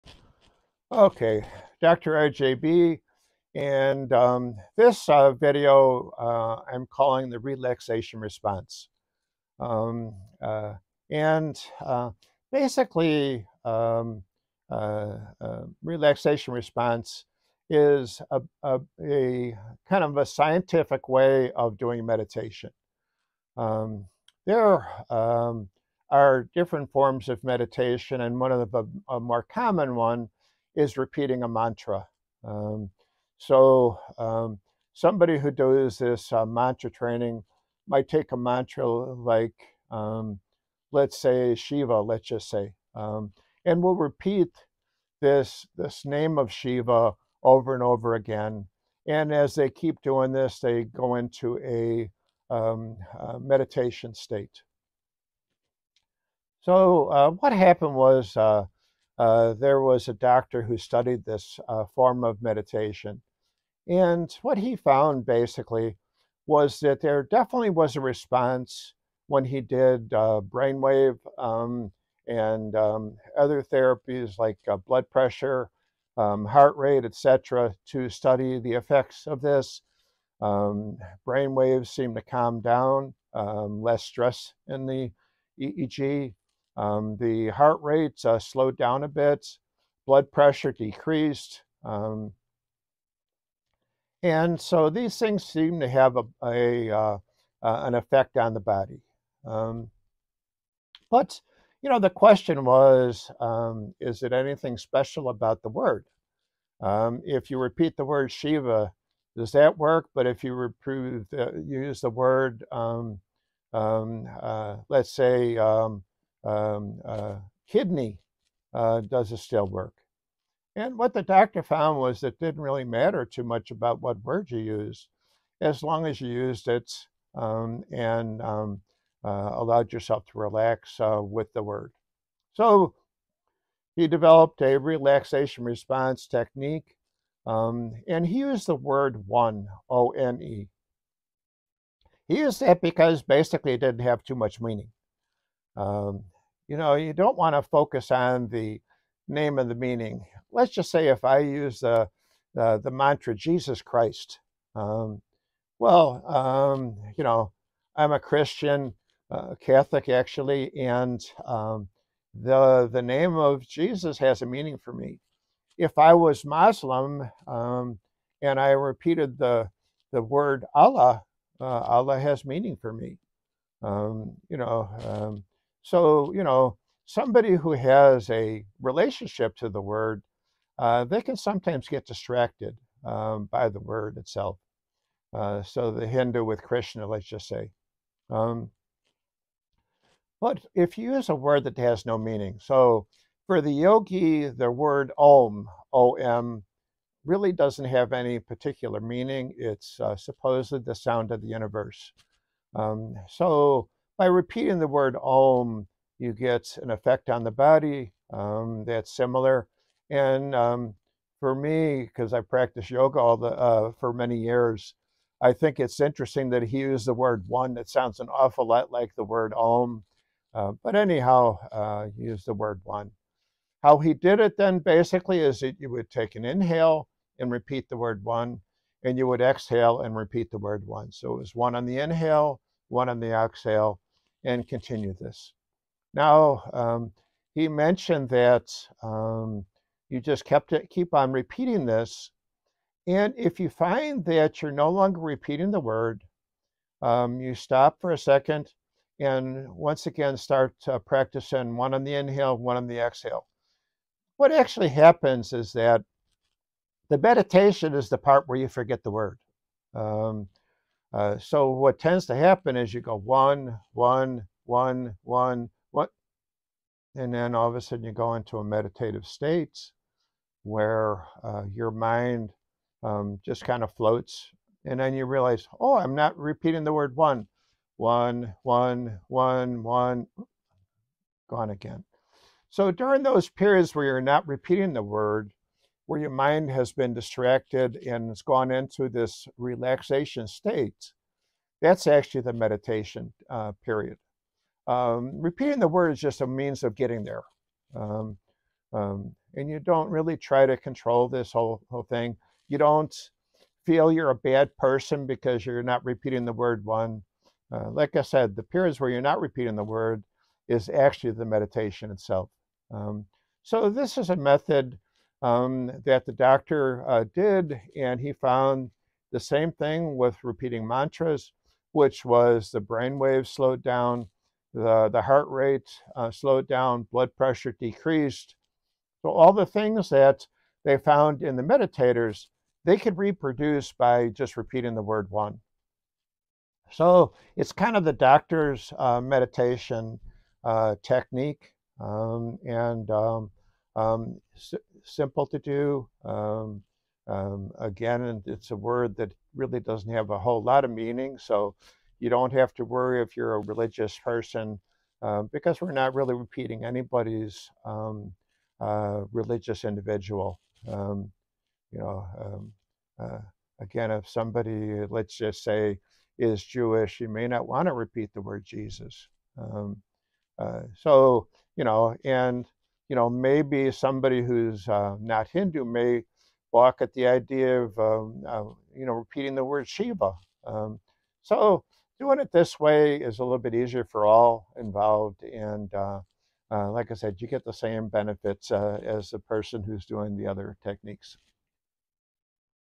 60: Eliminate Stress with the "Relaxation Response"
60: Eliminate Stress with the "Relaxation Response"

In this enlightening video, we explore the incredible potential of the "Relaxation Response" as a powerful tool to overcome stress and improve overall well-being. Join us as we delve into the ultimate stress buster and reveal the secrets to unlocking its extraordinary power.

Discover how the Relaxation Response helps us counter the negative effects of stress, promoting both physical and mental relaxation. Learn invaluable techniques and scientifically backed methods to trigger this response, allowing you to experience profound calmness and serenity in your daily life.

Our expert guides you through effective strategies to eliminate stress, teaching you practical steps to activate the Relaxation Response whenever you need it the most. Understand the impact of stress on your body and mind, and empower yourself with the knowledge to conquer it.

Dive deep into the world of relaxation and experience the transformative benefits it brings. By embracing the Relaxation Response, you can enhance your focus, productivity, and overall happiness. Take control of stress and unlock your true potential today.

Don't miss out on this incredible opportunity to discover the ultimate stress buster. Experience the power of the Relaxation Response and embark on a journey towards a more peaceful and fulfilling life.

other music: doctorarjaybe owned